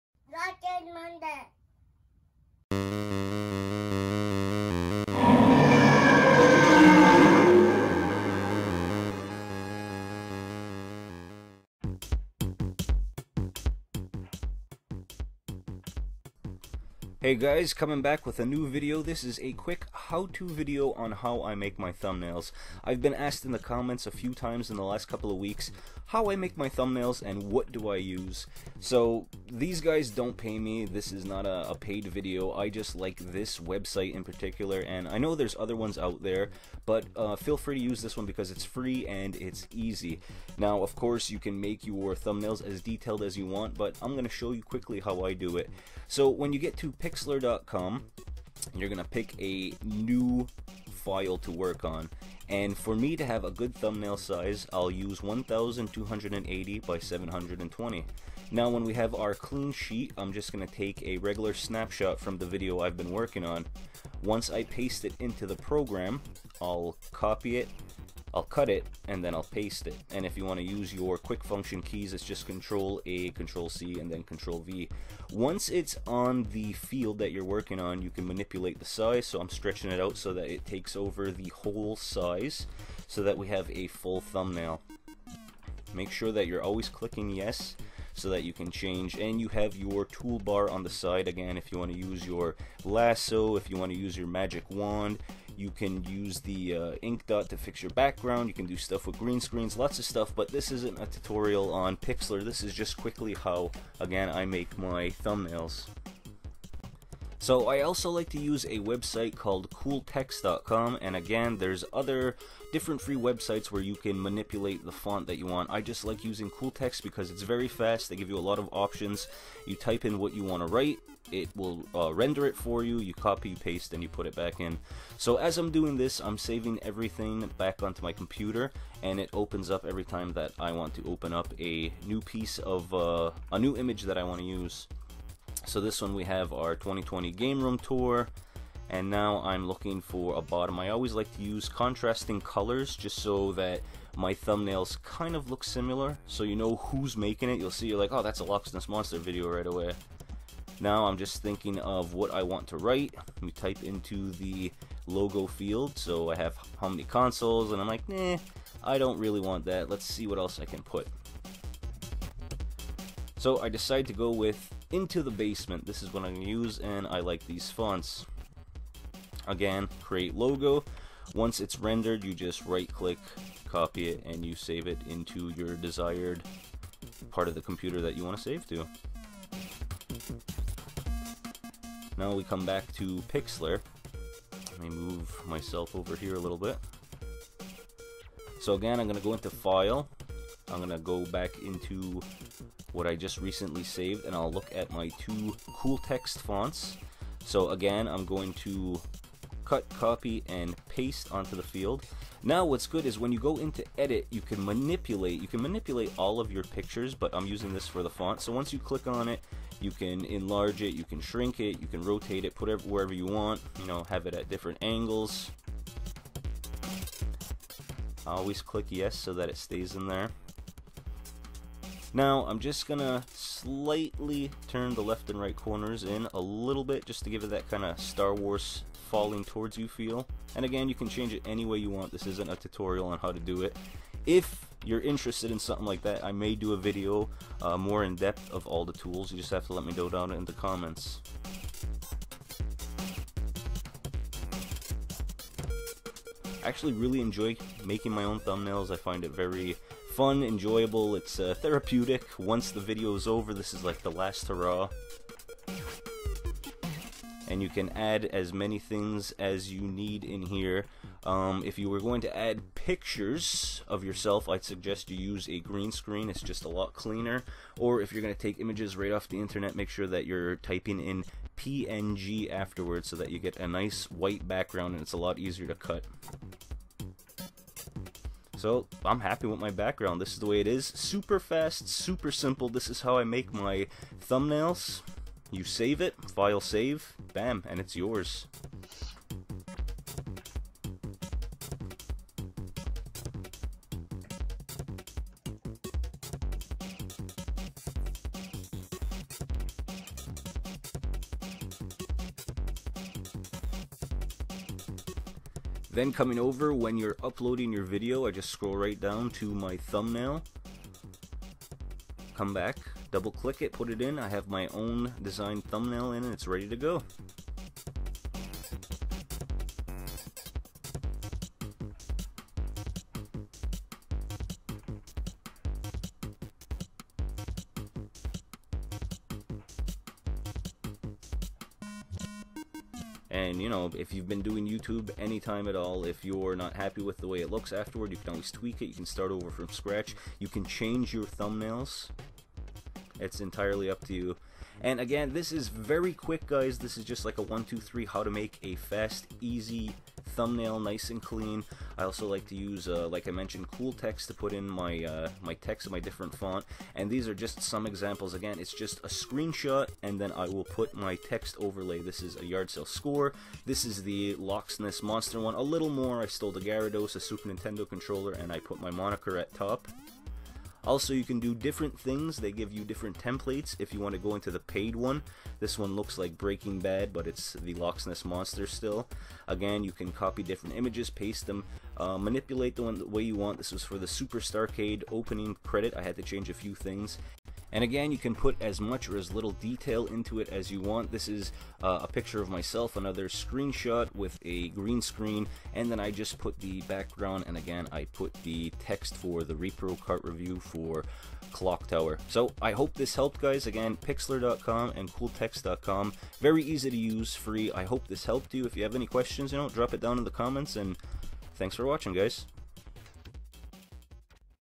HOW TO: EASY THUMBNAIL MAKER
Hey guys and gals, I've been asked a lot recently what I use and how I make my thumbnails for youtube videos. It's really easy, fun and fast. Here are my tips to make your thumbnail stand out.

the statute that might otherwise be